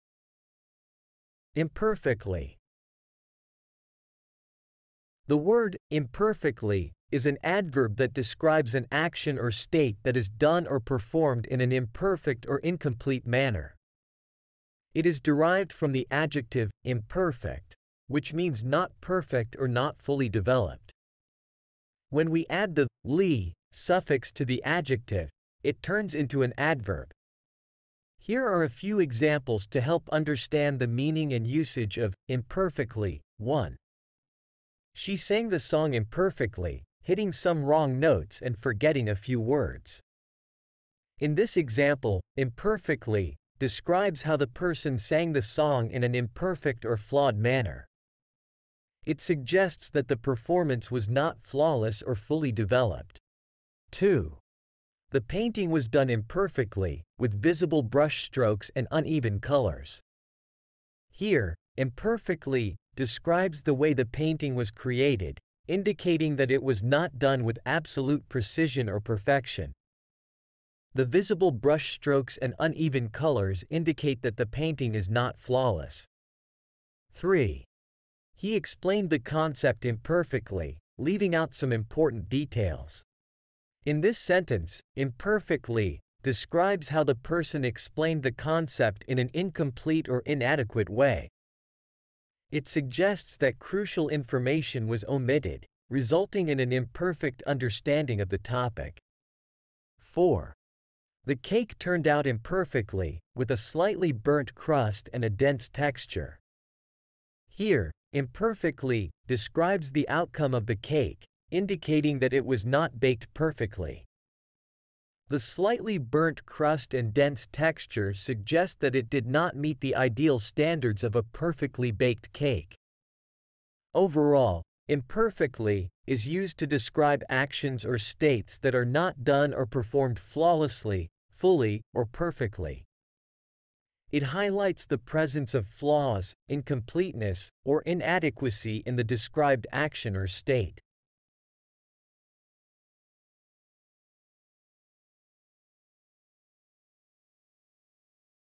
Imperfectly
The word "imperfectly" is an adverb that describes an action or state that is done or performed in an imperfect or incomplete manner. It is derived from the adjective "imperfect," which means not perfect or not fully developed. When we add the "-ly" suffix to the adjective, it turns into an adverb.
Here are a few examples to help understand the meaning and usage of "imperfectly":
1. She sang the song imperfectly, hitting some wrong notes and forgetting a few words.
   In this example, "imperfectly" describes how the person sang the song in an imperfect or flawed manner. It suggests that the performance was not flawless or fully developed.
2. The painting was done imperfectly, with visible brushstrokes and uneven colors.
   Here, "imperfectly" describes the way the painting was created, indicating that it was not done with absolute precision or perfection. The visible brushstrokes and uneven colors indicate that the painting is not flawless.
3. He explained the concept imperfectly, leaving out some important details.
   In this sentence, "imperfectly" describes how the person explained the concept in an incomplete or inadequate way. It suggests that crucial information was omitted, resulting in an imperfect understanding of the topic.
4. The cake turned out imperfectly, with a slightly burnt crust and a dense texture.
   Here, "imperfectly" describes the outcome of the cake, indicating that it was not baked perfectly. The slightly burnt crust and dense texture suggest that it did not meet the ideal standards of a perfectly baked cake.
Overall, "imperfectly" is used to describe actions or states that are not done or performed flawlessly, fully, or perfectly. It highlights the presence of flaws, incompleteness, or inadequacy in the described action or state.